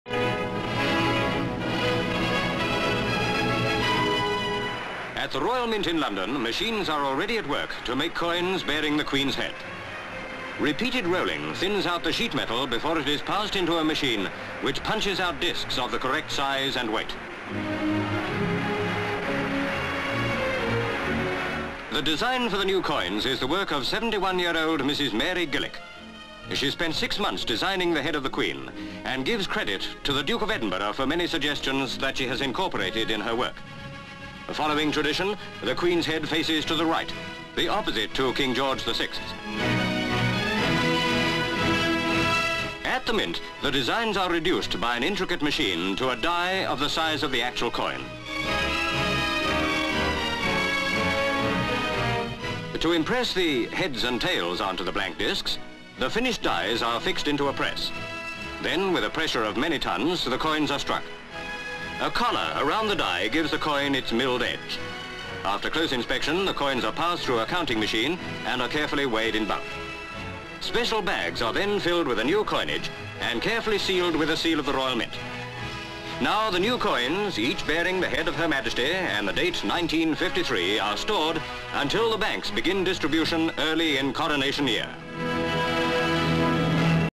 Queen's Coins Minted (1952)
Royal Mint.

Sv. Man pushing bars through rollers. CU. Reduced bars coming from rollers. LV. A stamping blanks from sheets of metal SV. Machine stamping blanks. & CU. Blanks falling into tray.

SV. Mrs. Gillick working on plaque of Queen's Head. CU.  Mrs. Gillick (Designer of coin). CU. Hand working on plaque. CU. Mrs. Gillick.

CU. Die reducing room Machine cutting die from artists original. CU. Machine tracing large die. SV. Man looking at reduction die. CU. Die being cut. LV. New coins being stamped out. CU. Machine stamped out coins. CU. New coins falling into tray. LV. Man sitting at counting and bagging machine. SCU. Coins running through machine. CU. Two bags filled from machine. CU. Bag of new coins being sealed. SV. Man placed bag of coins on truck loaded with bags and pulls truck away.

(Neg.) (Orig. 'D') (Uncut Neg. 'G') (Uncut Neg. 'H')
 FILM ID:44.51